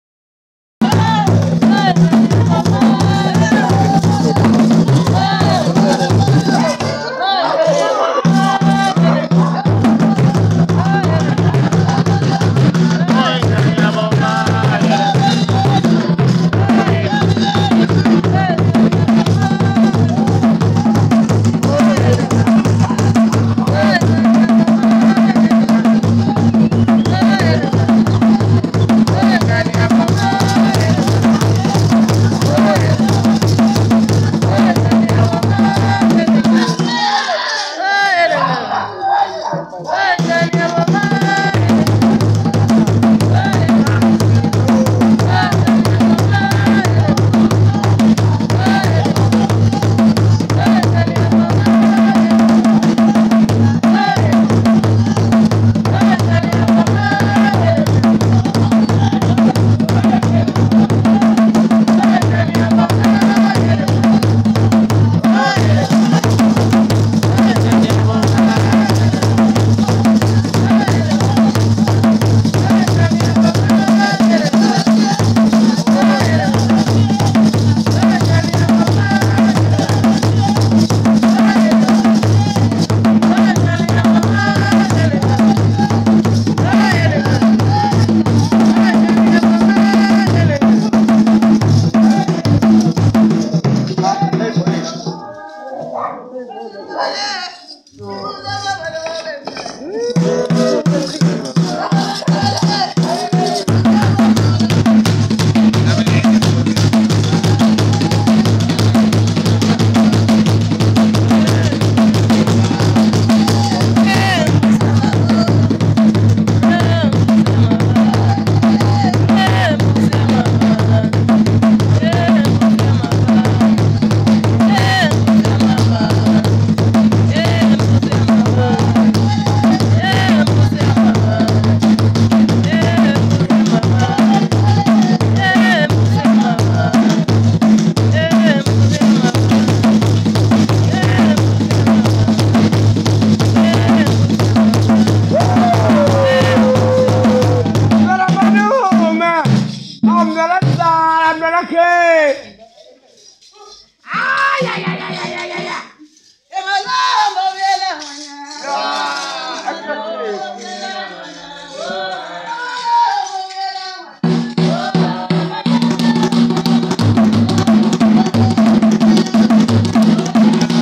Proud sangoma "inkani yaboma"sangoma
Umngoma raises other abangoma, evoking the sangoma within to rise, idlozi to take centre stage. The seasoned sangoma takes the initiates through their paces as an easing process, preparing them for their ultimate journey of intwaso